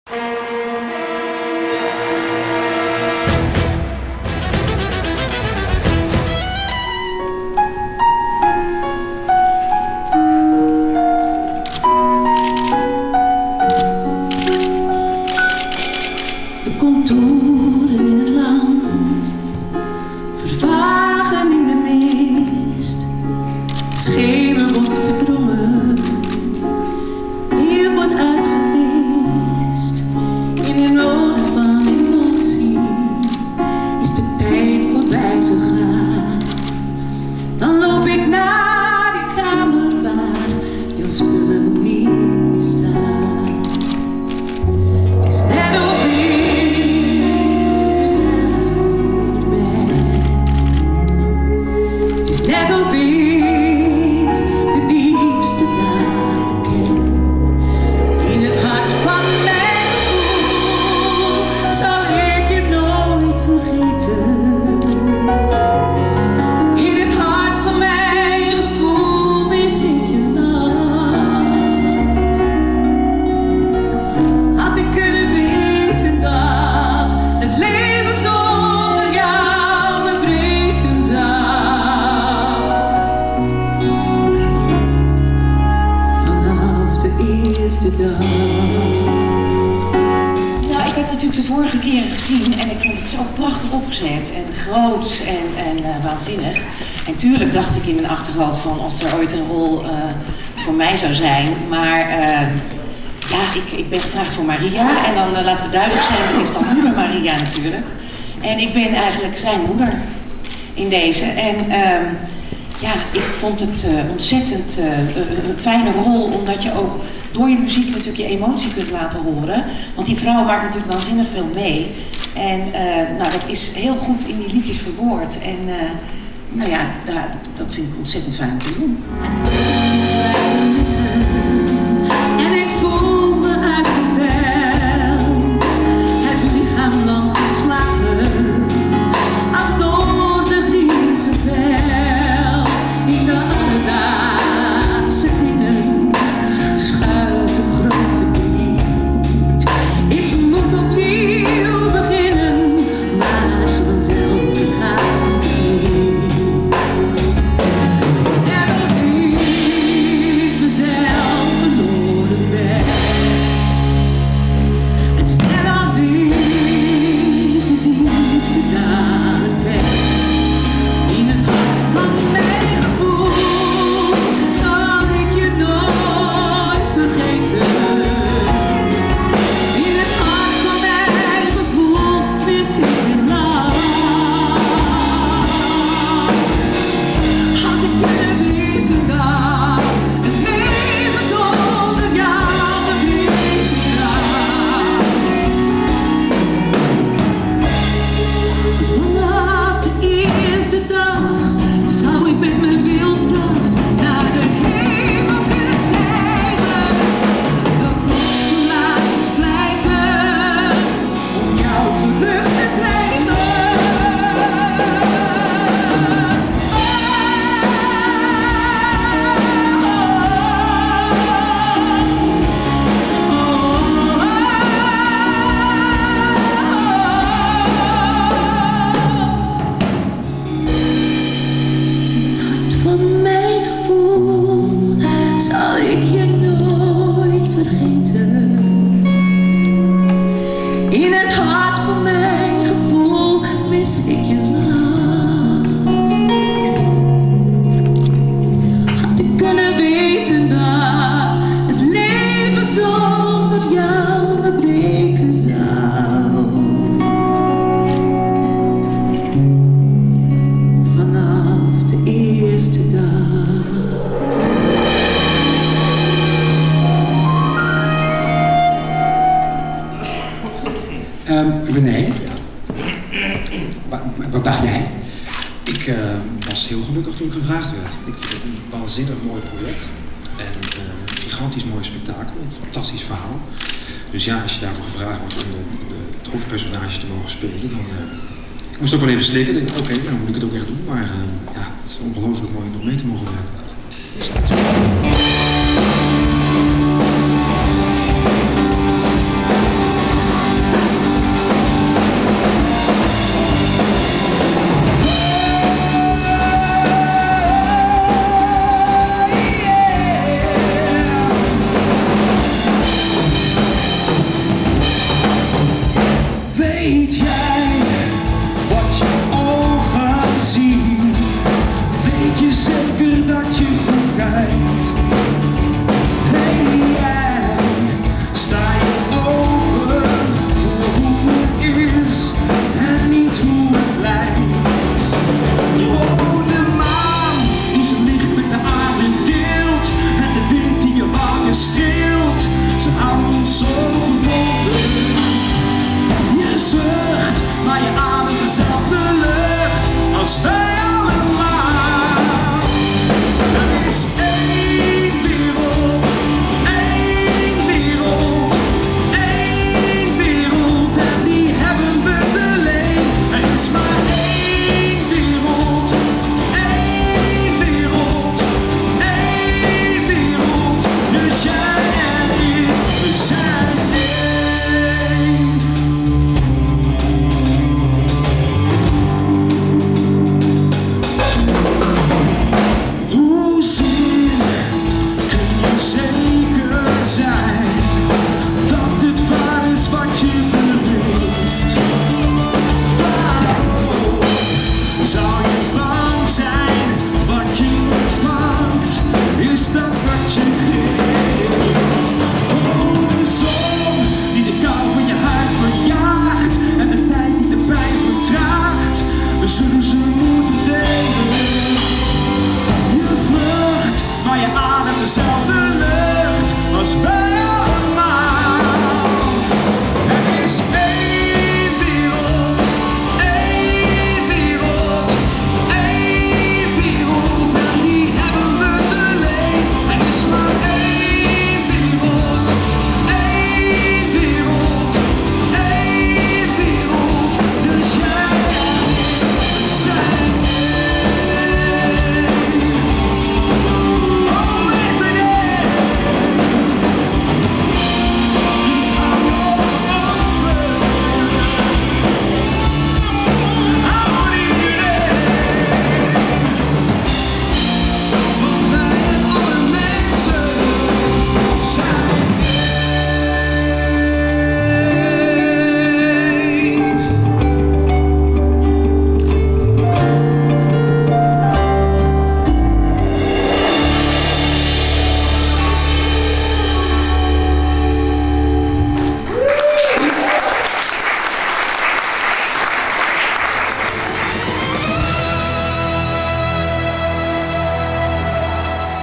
The Passion - perspresentatie - Den Haag 2013
Dit jaar met Rene van Kooten al Jezus en Anita Meijer als zijn moeder Maria.